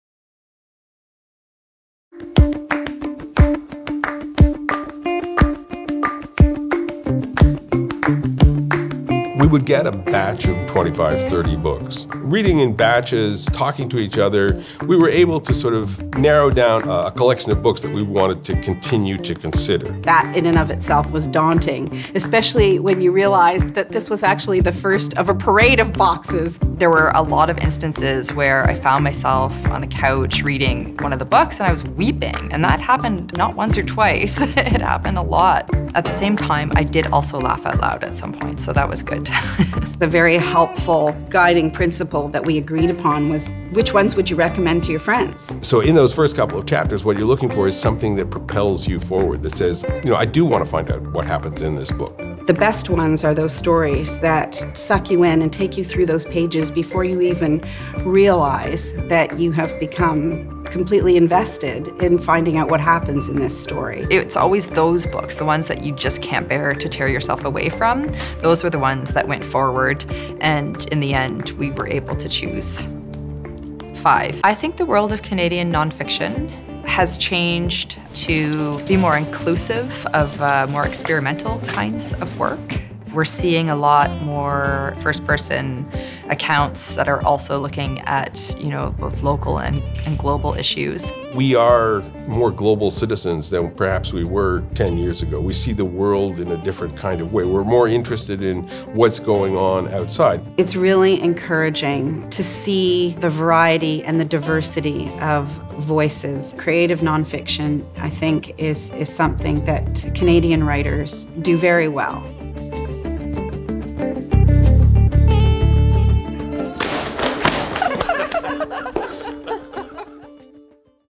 2016 Hilary Weston Writers' Trust Prize for Nonfiction Jury Talks
Prize jurors Carolyn Abraham, Stephen Kimber, and Emily Urquhart talk about their experience adjudicating the 2016 Hilary Weston Writers' Trust Prize for Nonfiction and their thoughts on contemporary Canadian nonfiction writing.